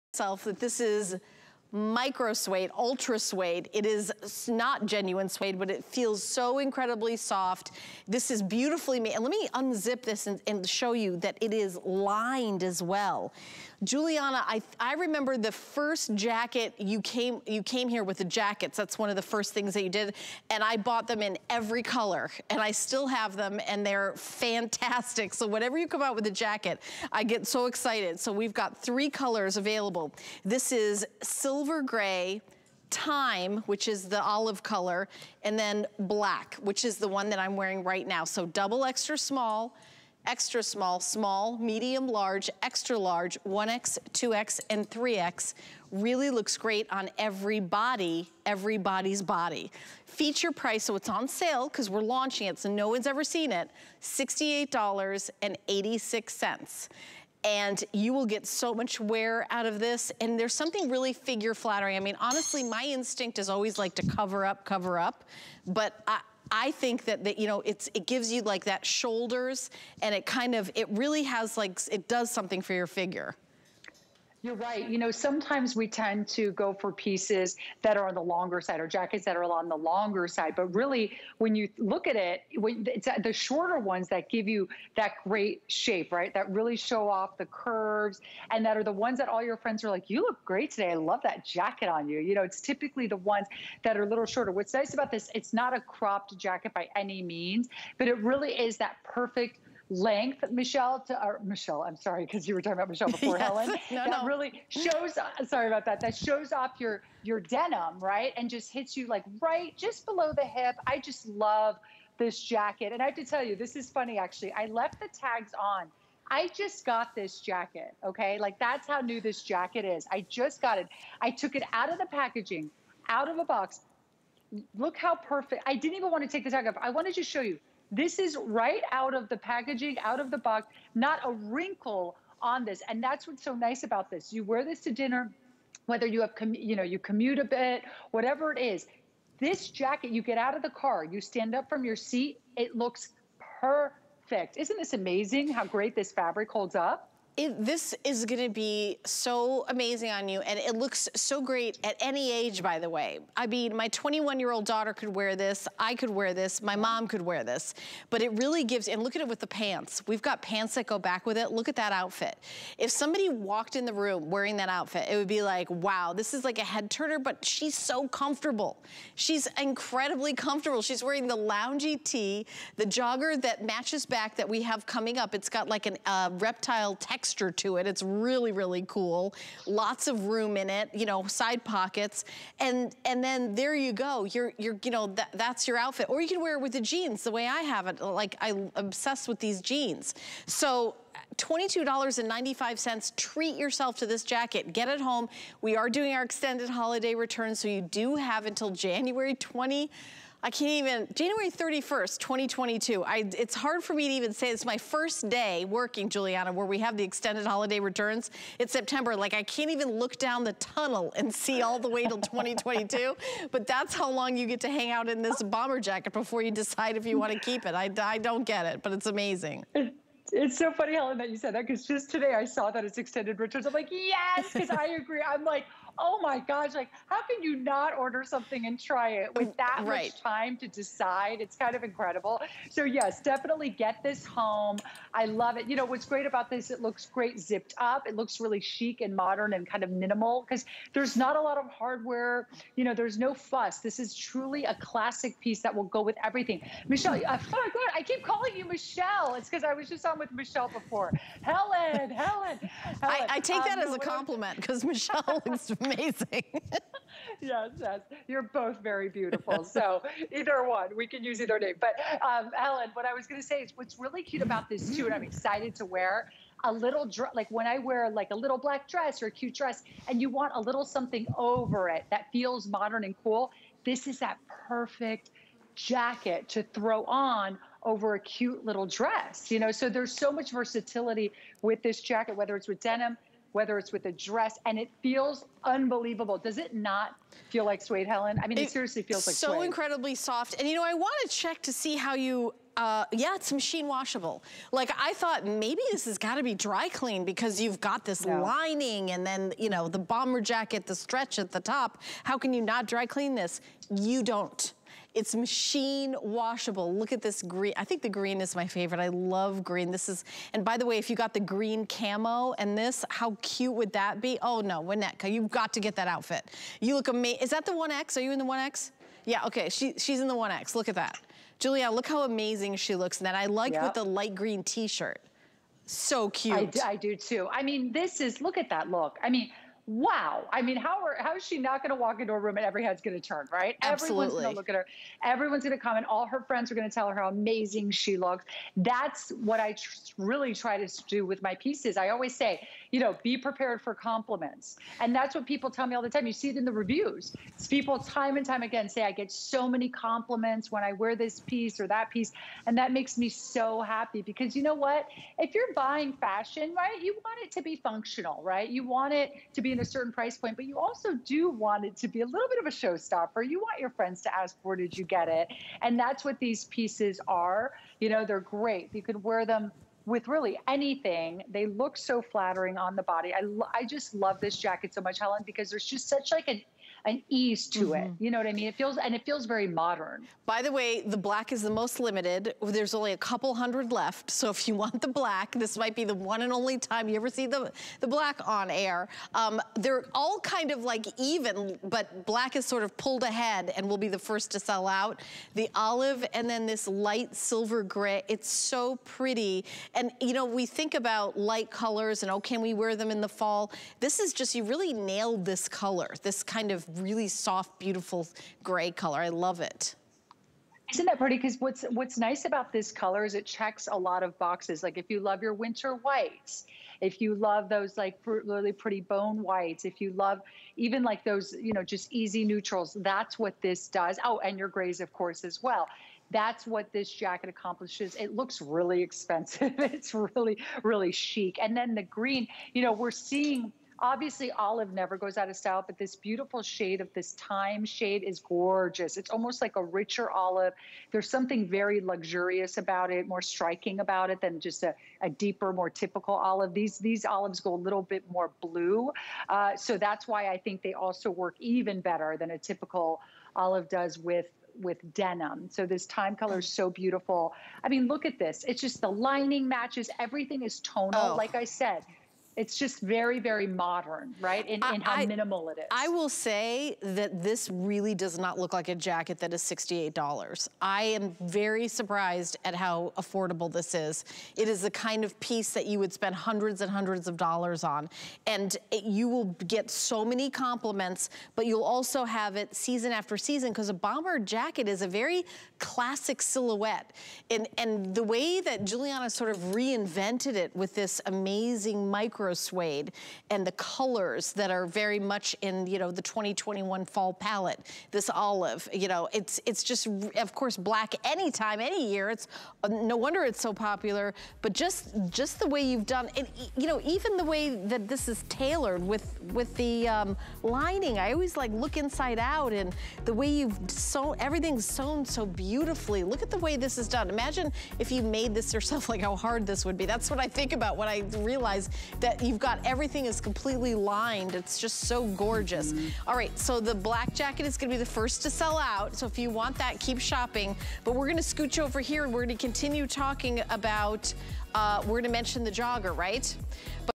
G by Giuliana Ultrasuede Bomber Jacket
Get the look and feel of suede without the weight (or hefty price tag). Giuliana's stretch faux suede fabric is lightweight and completely luxurious in this classic bomber jacket silhouette.